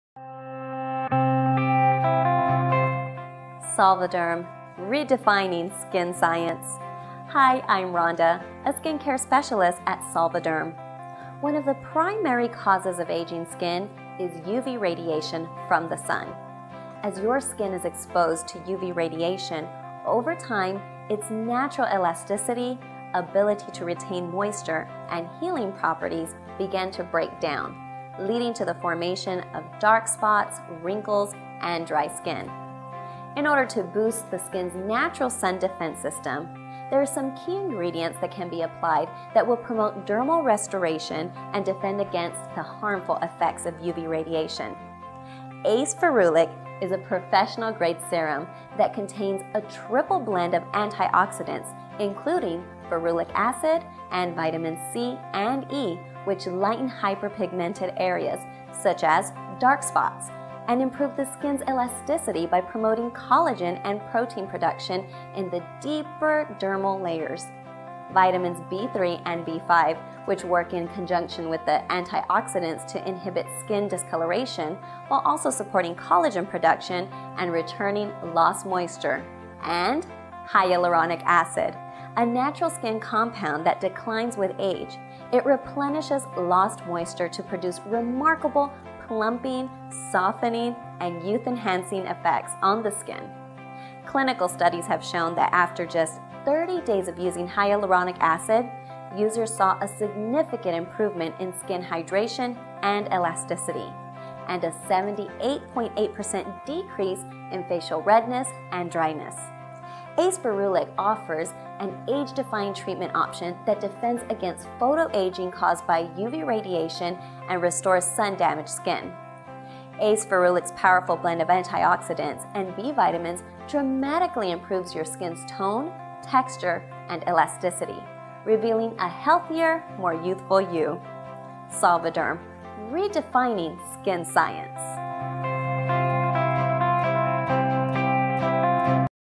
Solvaderm ACE-Ferulic - Advanced Skin Rejuvenating Serum
SOLVADERM'S ACE-FERULIC is an advanced rejuvenating serum that promotes dermal restoration and provides a powerful defense against skin aging due to sun exposure and environmental stressors.

Video Transcript

Hi, I’m Rhonda, a skincare specialist at Solvaderm.

One of the primary causes of aging skin is UV radiation from the sun. As your skin is exposed to UV radiation, over time, its natural elasticity, ability to retain moisture, and healing properties begin to break down, leading to the formation of dark spots, wrinkles, and dry skin. In order to boost the skin's natural sun defense system, there are some key ingredients that can be applied that will promote dermal restoration and defend against the harmful effects of UV radiation.

ACE Ferulic is a professional-grade serum that contains:

A triple blend of antioxidants including Ferulic Acid and Vitamins C and E which lighten hyper pigmented areas such as dark spots, and improve the skin’s elasticity by promoting collagen and protein production in the deeper dermal layers

Vitmins B3 and B5 which work in conjunction with the antioxidants to inhibit skin discoloration, while also supporting collagen production and returning lost moisture

AND

Hyaluronic Acid, a natural skin compound that declines with age, it replenishes lost moisture to produce remarkable plumping, softening, and youth-enhancing effects on the skin

Results

Clinical studies have shown that:

After just 30 days of using Hyaluronic acid, users saw a significant improvement in skin hydration and elasticity, and a 78.8% decrease in facial redness and dryness.

Conclusion

ACE Ferulicoffers an age-defying treatment option that defends against photo aging caused by UV radiation and restores sun damaged skin. ACE Ferulic’s powerful blend of antioxidants and B-vitamins dramatically improve your skin’s tone, texture, and elasticity, revealing a healthier, more youthful you.

Solvaderm: Redefining Skin Science